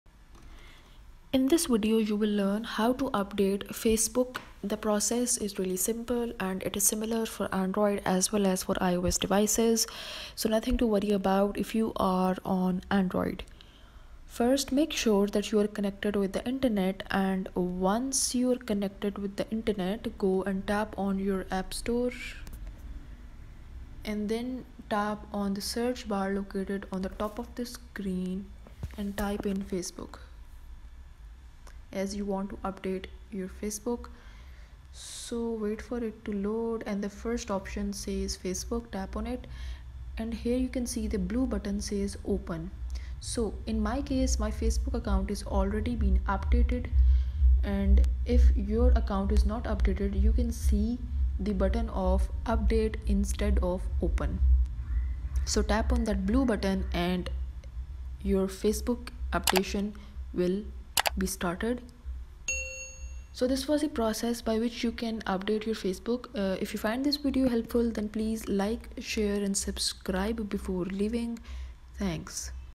How to Update Facebook App iPhone and Android (Quick & Simple)
Learn How to Update Facebook App. It is simple process to update facebook app on iPhone and Android, simply follow this video.

0:00 Intro
0:05 Update Facebook App iPhone and Android